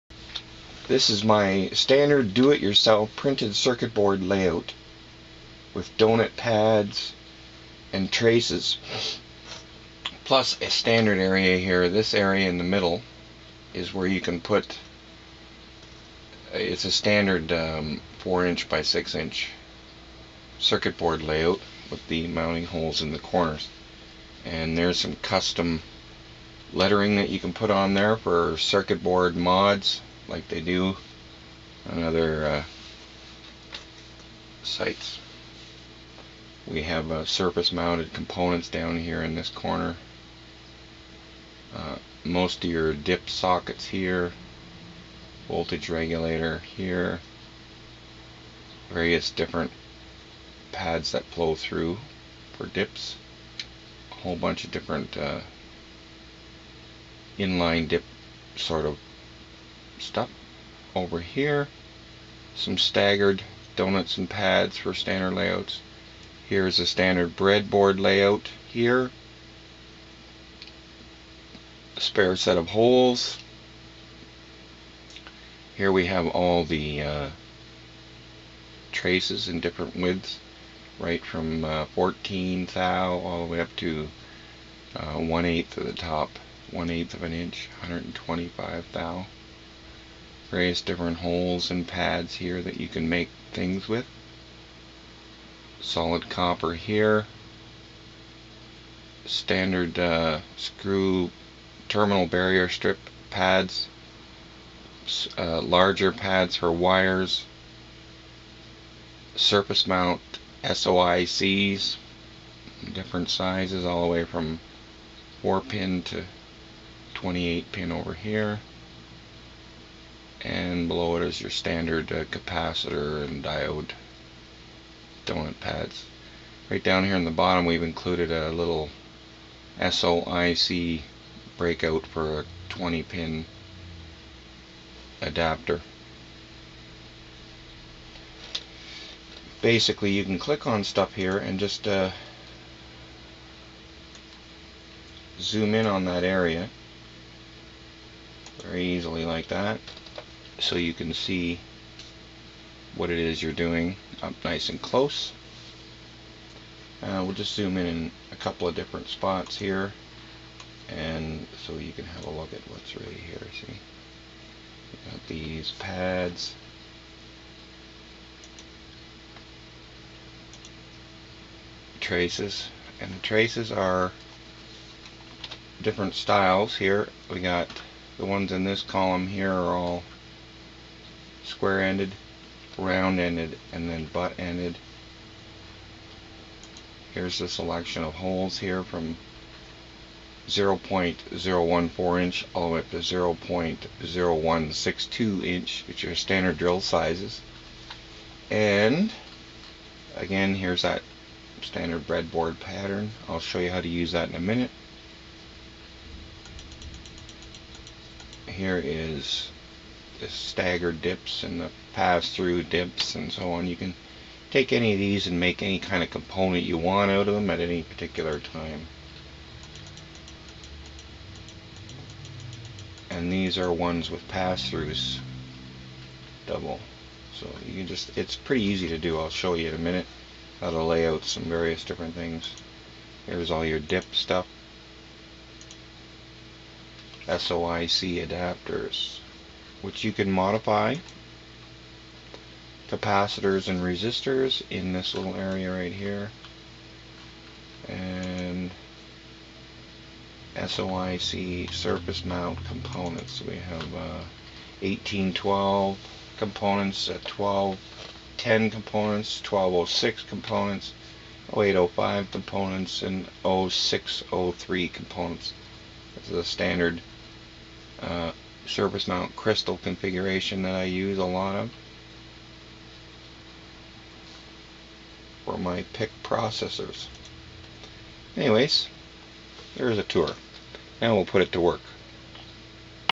Simple Design Method PC Board PCB Artwork Layout DIY
The very simple method I have used for years to design compact printed circuit board layouts. I get PDF or file printed at copy shop for few bucks then use the positive photosensitive resist etch method. You don't need expensive or limited software. I have made board 12" x 18" using the same method. Make multiple board layouts on one standard sheet.